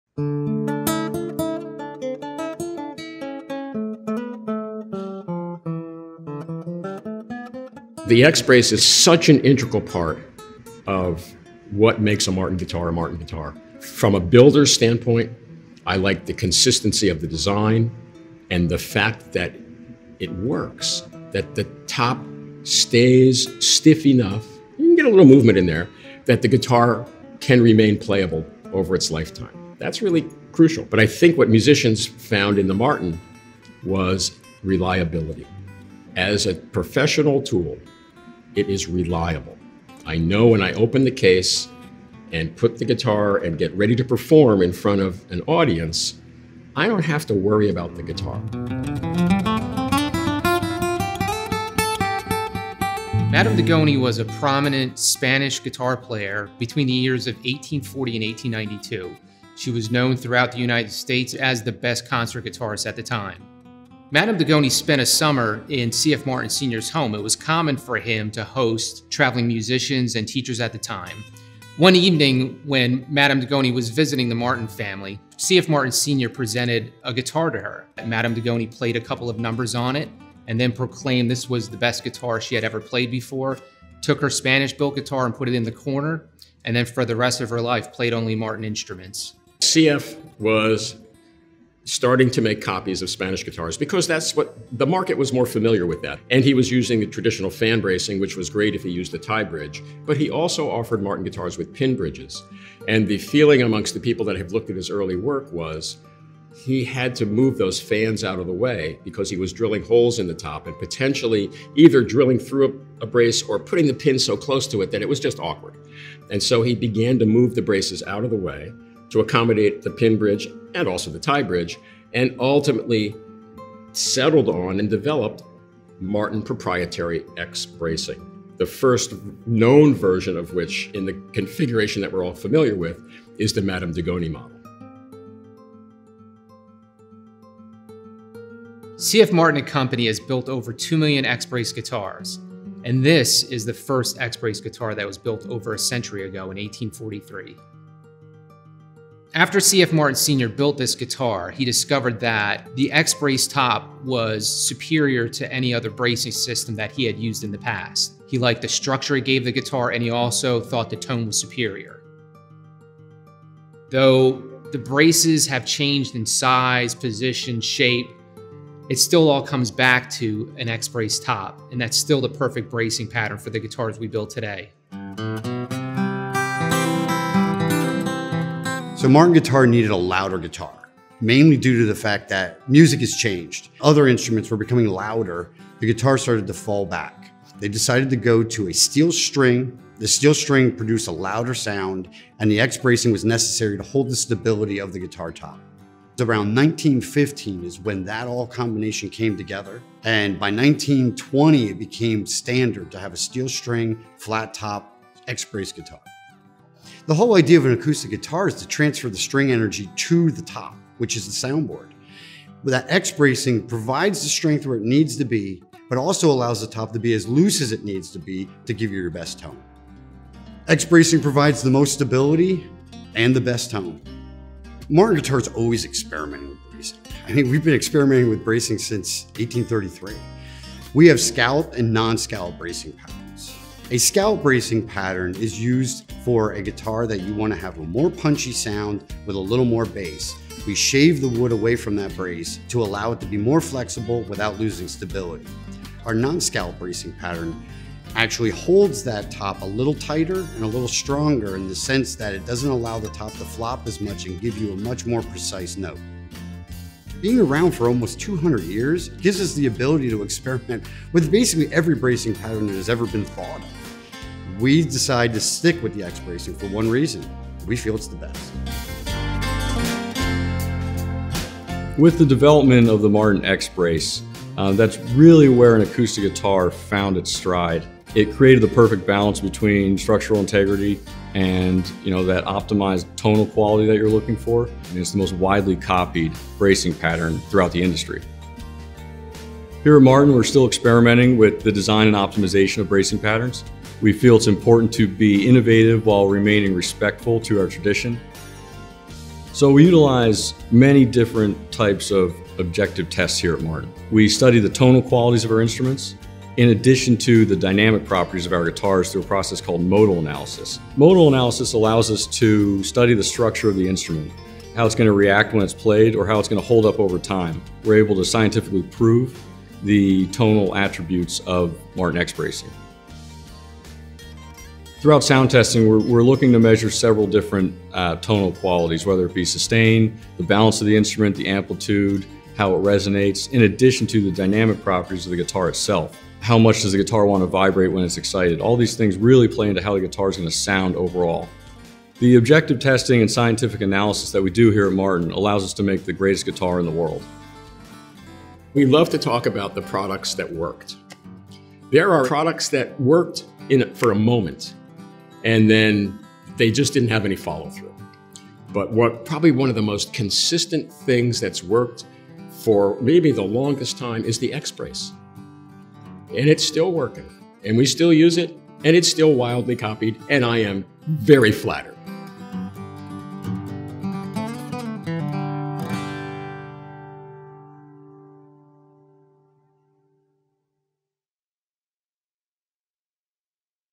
Martin X Bracing
A look into the history and origin of X Bracing.